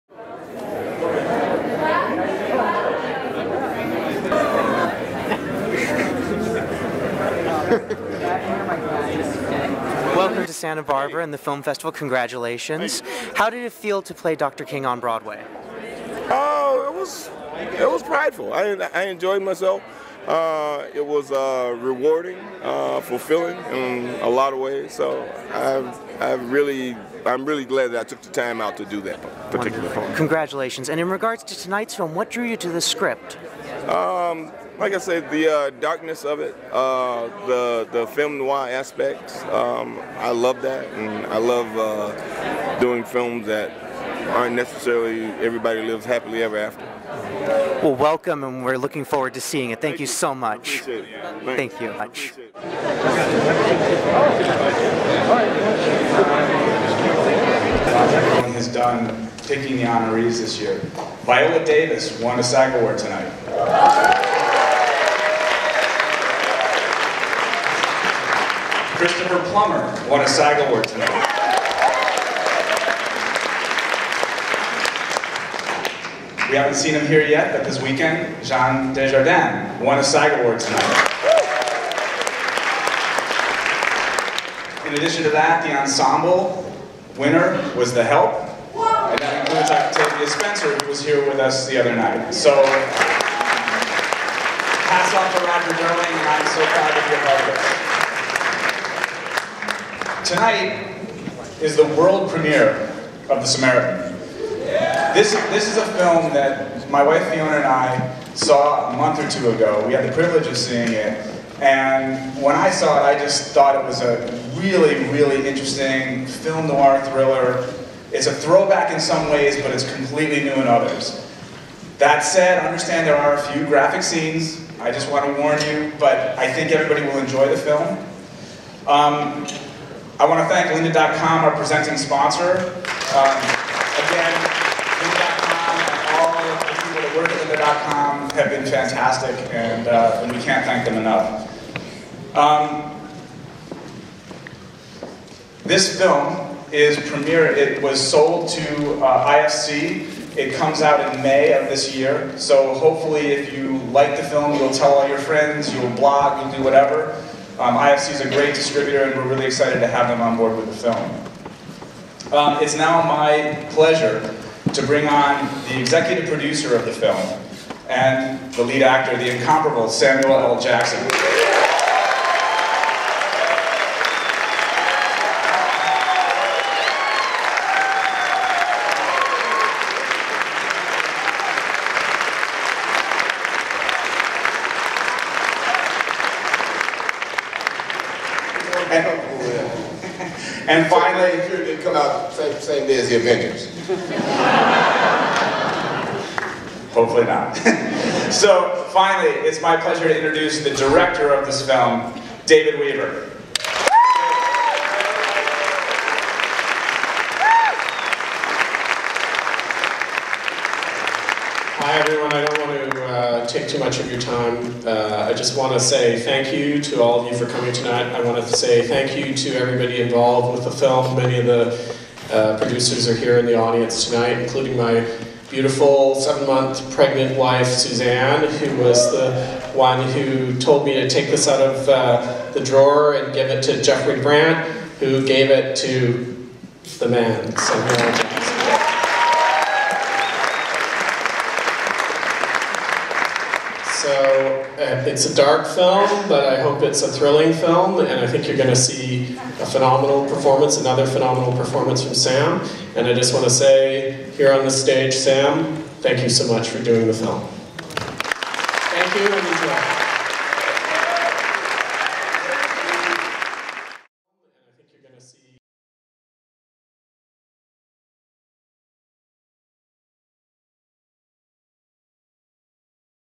SBIFF 2012 - Samuel L. Jackson in "The Samaritan"
Samuel L. Jackson stars in "The Samaritan", a gritty modern noir about an ex con being sucked back into the seedy underworld he's tried so long to escape. Attending the gala premiere at SBIFF 2012, Mr. Jackson took a moment to speak with us about his new film and his recent turn on Broadway as Dr. Martin Luther King Jr.